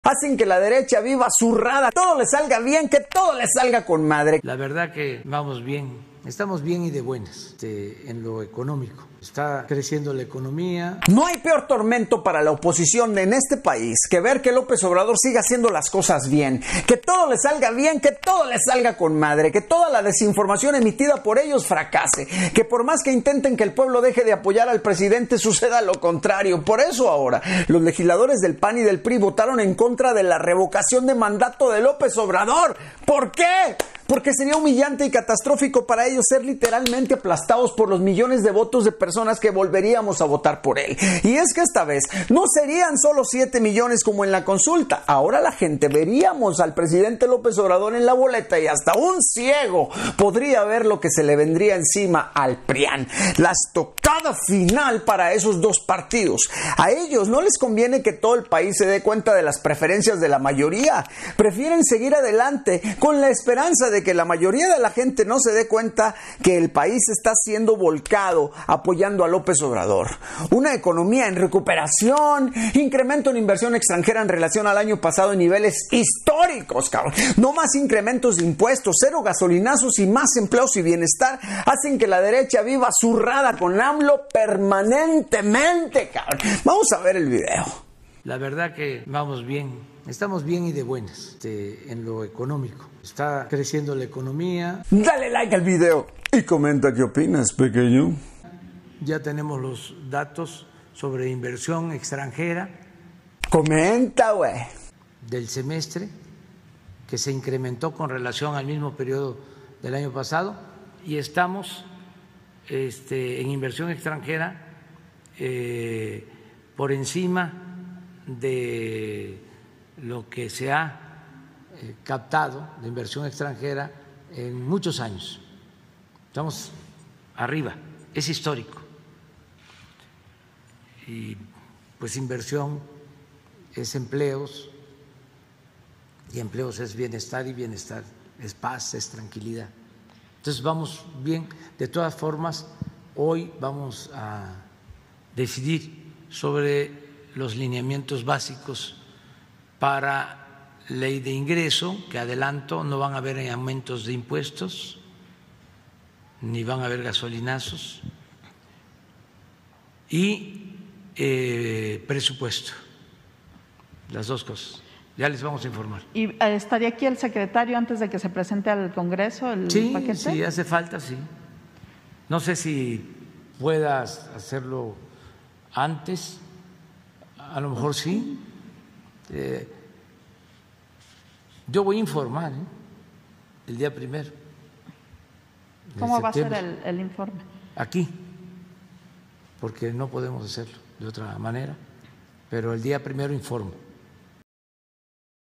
La Derecha vive Surrada con AMLO por que todo le sale bien
La Derecha vive Surrada con AMLO por que todo le sale bien. La economia crece, la inversion extranjera crece, las remesas crecen, la gasolina y el peso se mantienen, ah, y seguimos con cero deuda.

 las noticias como son
wichasalazar

wicha_salazar

wichalasnoticiascomoson